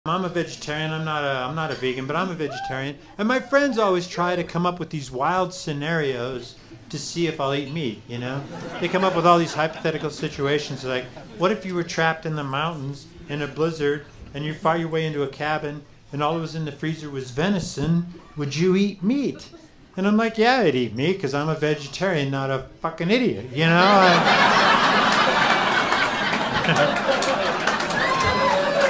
John Novosad
John Novosad, a.k.a. Hippieman, is a stand-up comedian based out of the Comedy Works in Denver, Colorado. He’s performed in clubs, theatres and bars across the country for over thirty years. John grew up in Boulder, Colorado.

Making his network television debut on The Late Late Show with Craig Ferguson, John followed that up with a second performance on the Ferguson show. He’s also appeared on Bryon Allen’s nationally syndicated Comedy.TV.